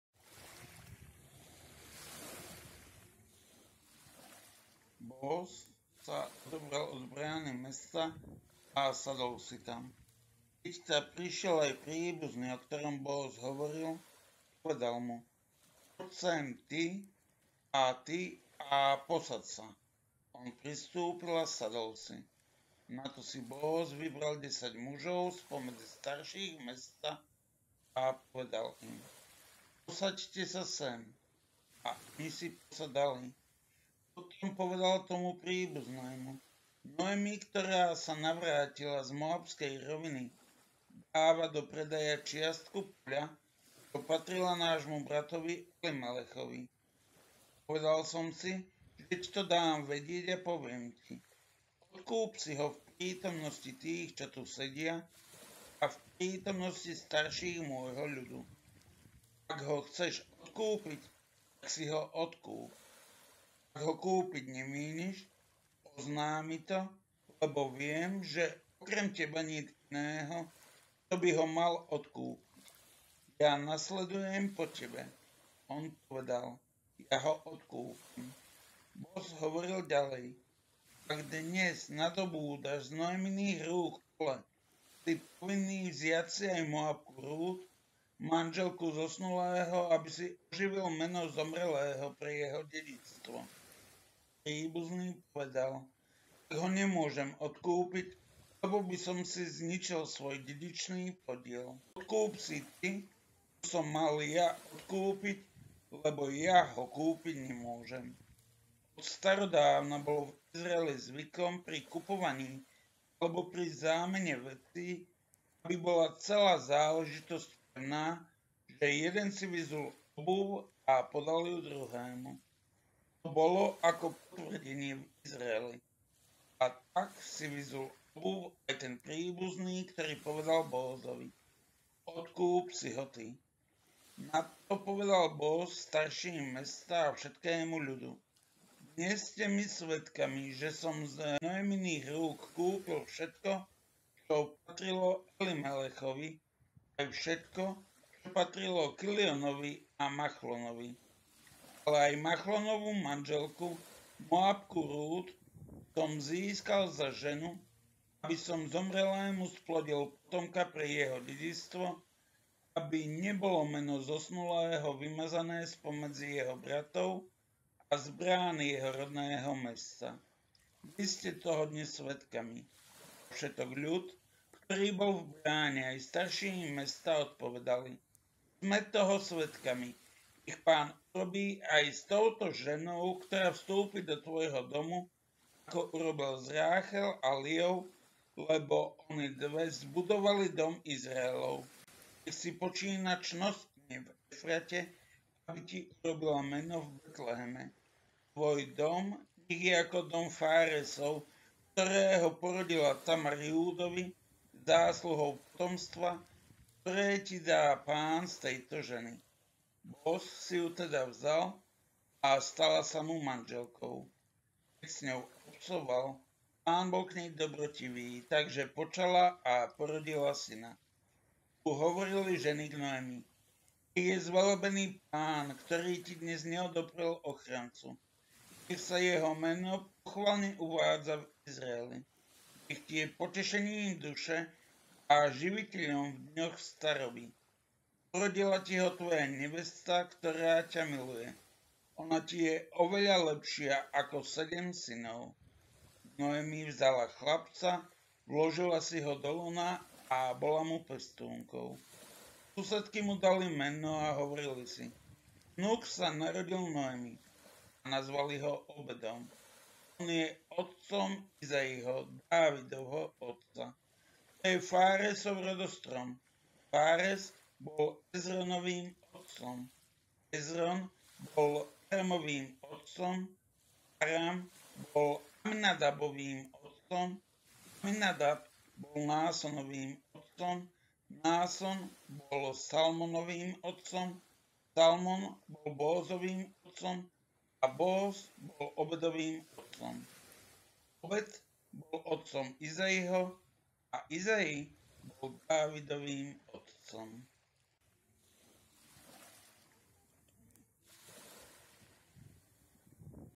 Rút Boázovou manželkou: Boáz odkúpil Noemino pole a nadobudol právo na manželstvo s Rút
Kniha Rút

Rút Boázovou manželkou (4,1-17):
Boáz odkúpil Noemino pole a nadobudol právo na manželstvo s Rút (4,1-12)
Rút Boázovou manželkou (4,13-17).
Dodatok: rodokmeň od Peresa [Fáresa] po Dávida (4,18-22).